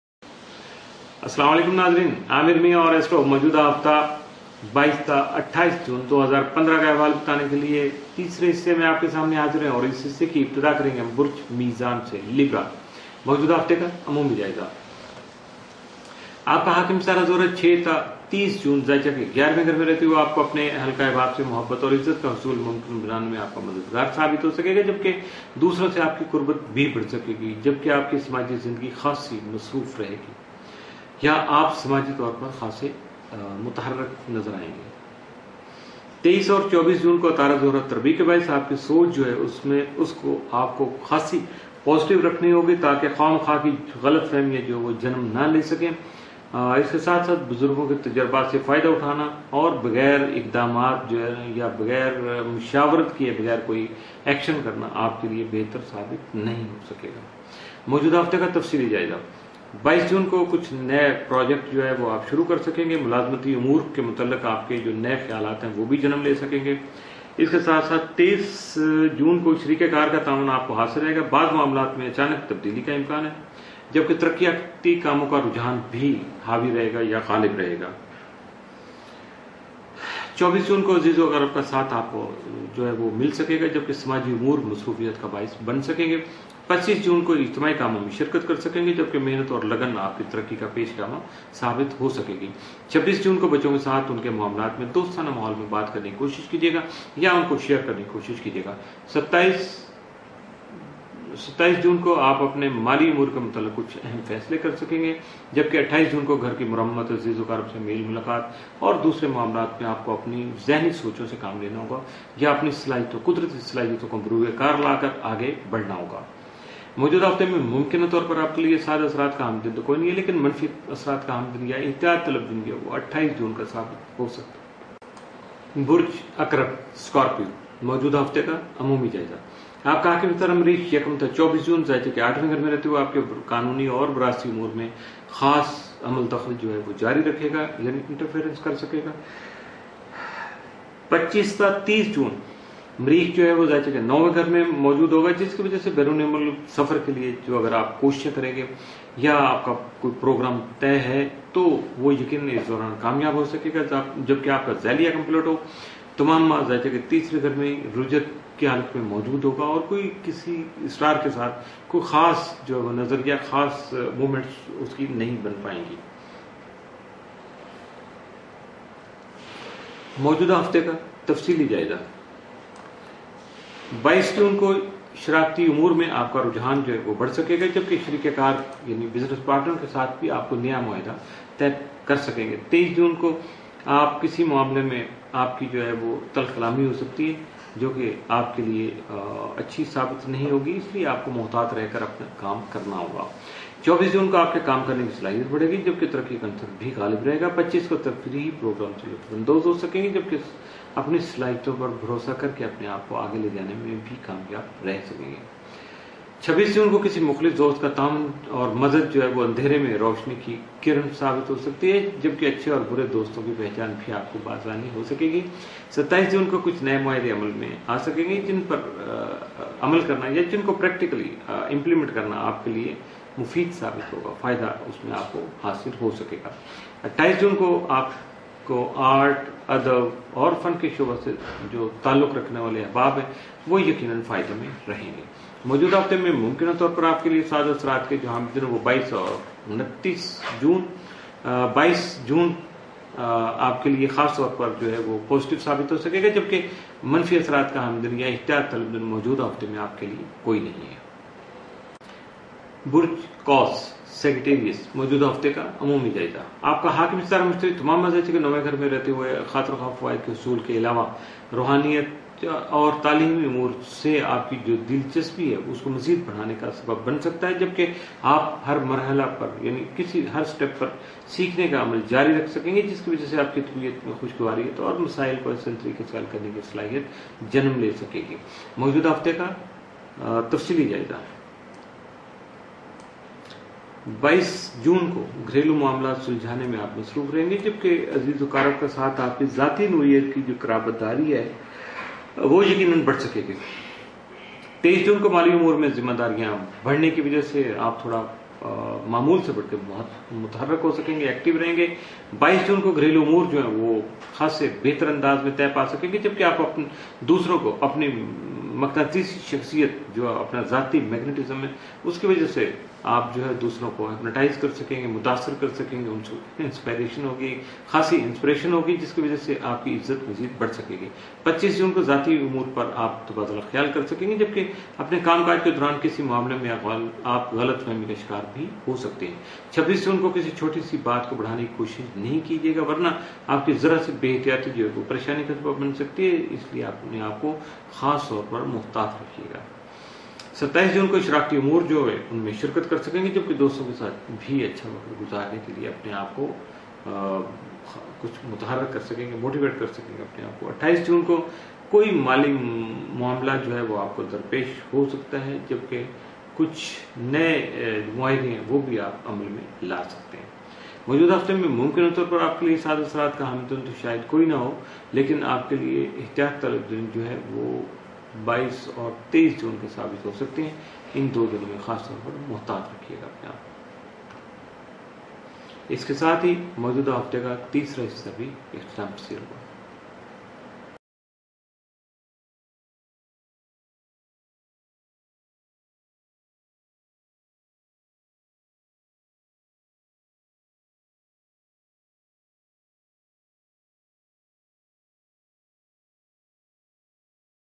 Weekly Urdu Horoscope from 22 to 28 June 2015 Part 3
Weekly Urdu Horoscope by Aameer Mian from 22 to 28 June 2015 Part 3 for Libra Scorpio & Sagittarius